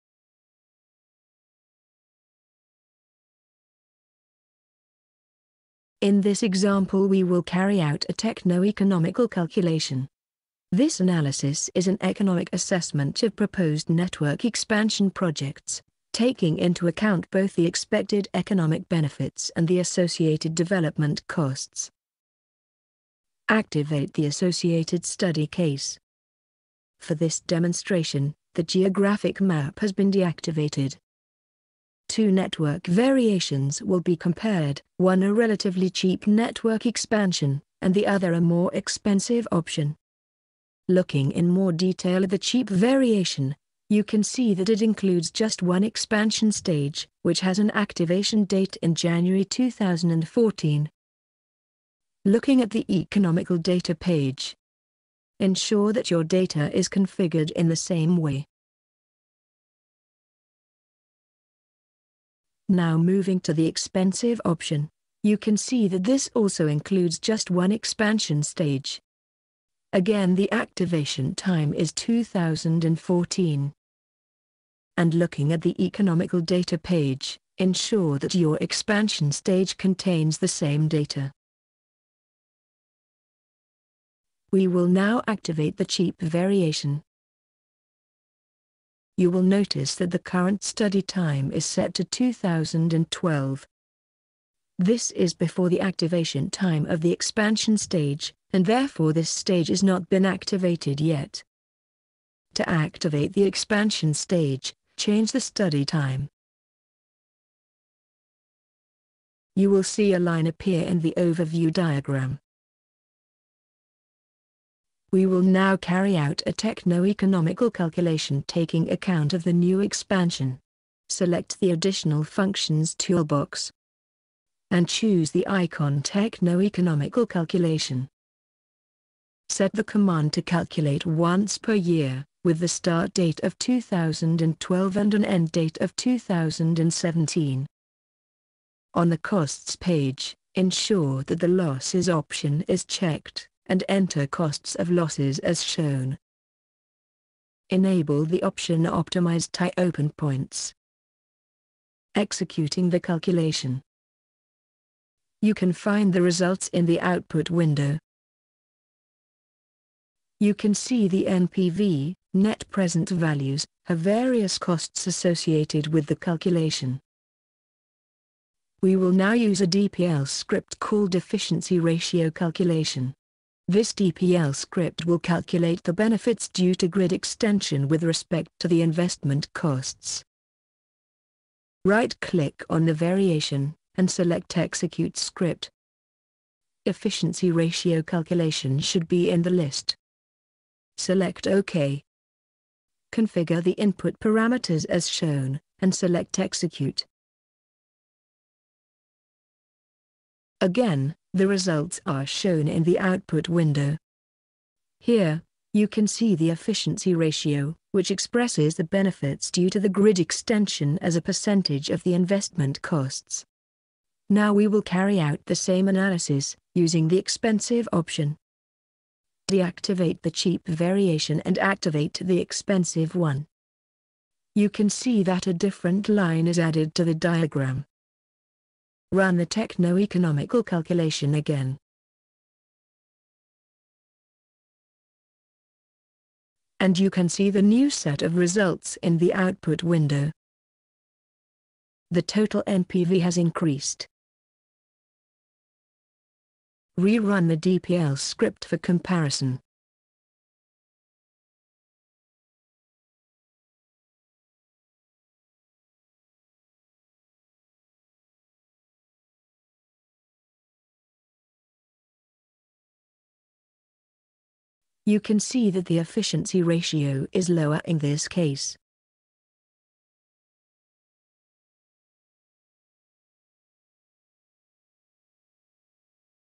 PowerFactory - MV Distribution Network - Techno Economical Calculation
A techno-economical calculation is carried out to compare the economical implications of two alternative technical solutions.